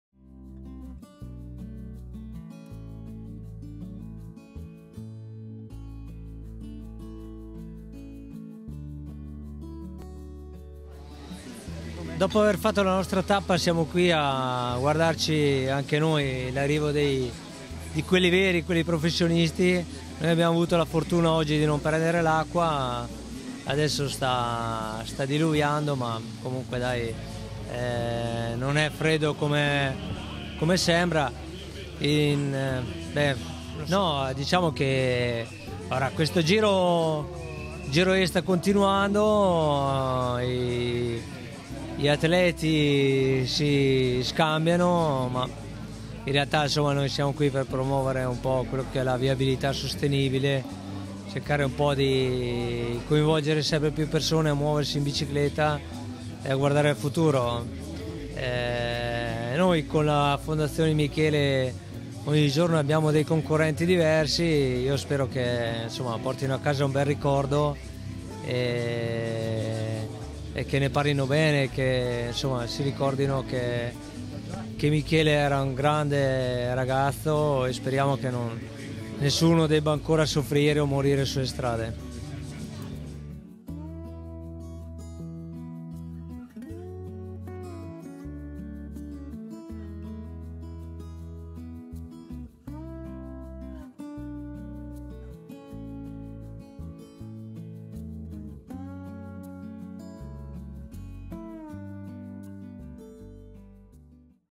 Gilberto Simoni - Giro E 2020 11° Tappa: San Marino - Cesenatico
Intervista a Gilberto Simoni -  Giro E 2020 11° Tappa: San Marino - Cesenatico - 15 ottobre 2020 -
 A cura di Antonio Abbruzzese